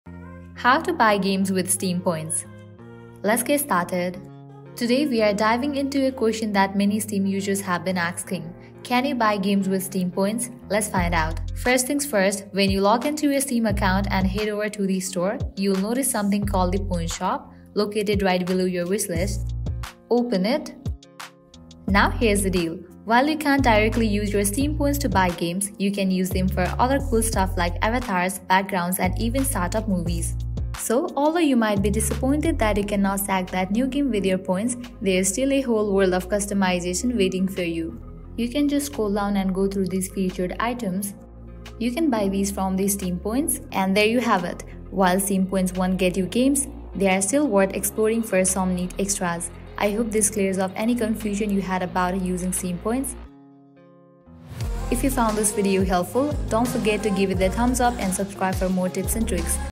How To Buy Games With Steam Points (LATEST GUIDE)
🎮💳 Purchasing Games with Steam Points (Latest Guide)

Eager to utilize your Steam Points for purchasing games? Our latest guide provides step-by-step instructions on how to buy games using Steam Points, ensuring you can make the most of your rewards and enjoy your favorite titles hassle-free.

In this guide:
0:00 - Introduction
0:05 - How To Buy Games With Steam Points (LATEST GUIDE)

Unlock the process of purchasing games with Steam Points with our easy-to-follow instructions. Whether you're browsing the Steam store or accessing your Points Shop, we'll walk you through the steps to redeem your points and acquire new games for your library.

Start using your Steam Points to expand your gaming collection with our latest guide!